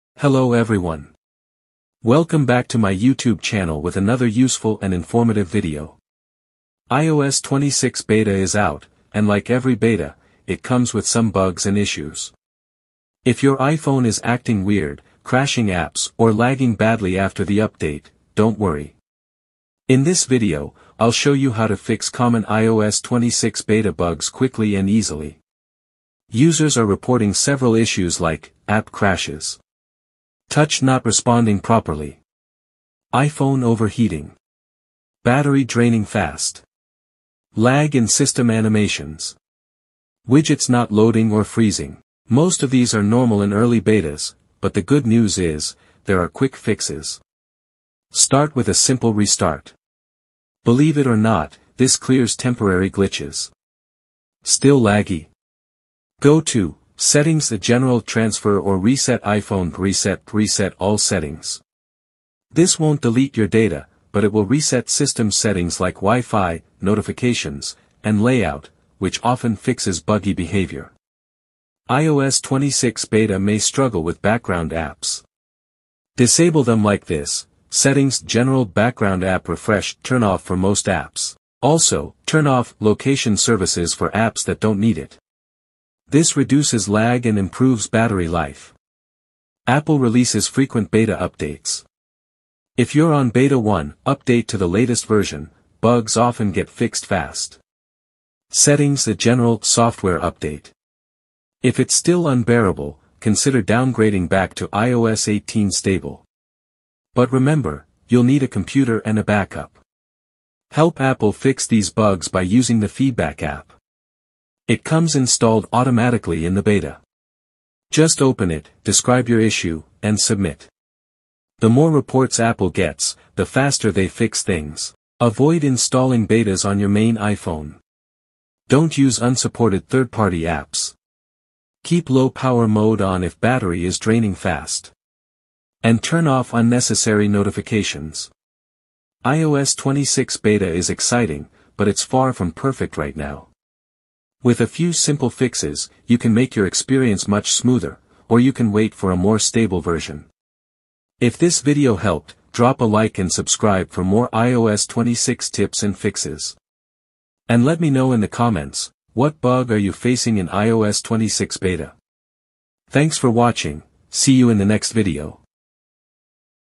iOS 26 Beta Bugs | How to Fix Bug issues on iOS 26 Beta
Facing bugs or glitches after installing iOS 26 Beta on your iPhone or iPad? In this video, I’ll walk you through the most common iOS 26 Beta problems and how to fix them quickly. From app crashes to battery drain and lag, I’ll show you tested solutions to get your device running smoothly again. Don’t let beta bugs ruin your experience — watch now and fix them all!